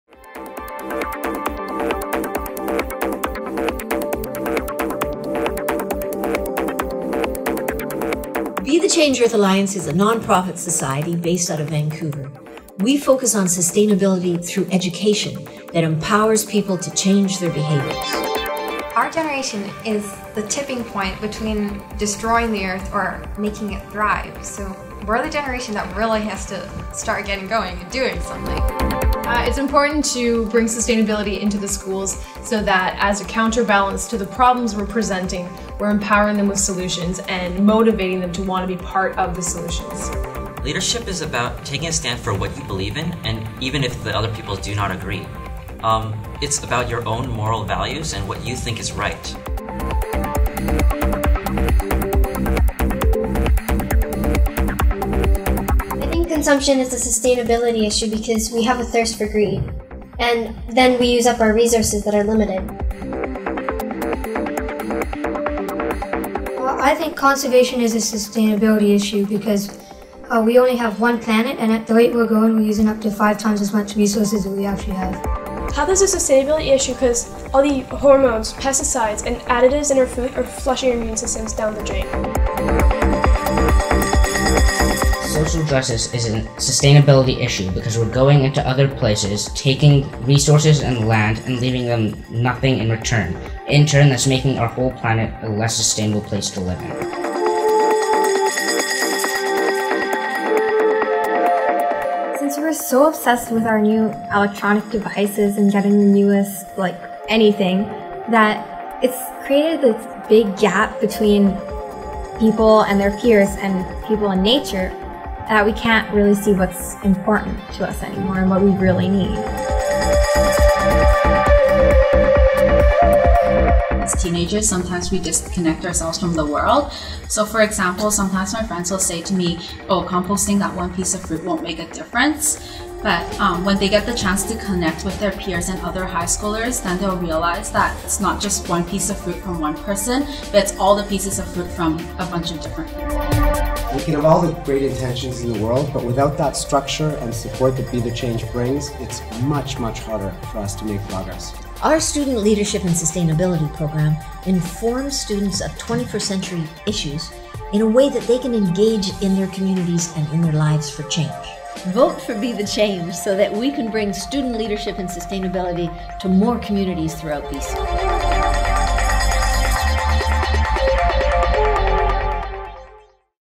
Student Leadership in Sustainability - Be The Change Earth Alliance
Thank you to Pull Focus Film, and the supportive Killarney, Kitsilano & Point Grey Teachers -Denise North, Rob Furguson, Dave Derpak, Raphaelle Dumais & Irfan Sheik
and the Killarney Environment Club & Kitsilano FI Ecology Class (all using SLS materials).
We had lots of great footage that didn't make it into this short clip - thank you all for great input!

"Be the Change you wish to see in the world" - Gandhi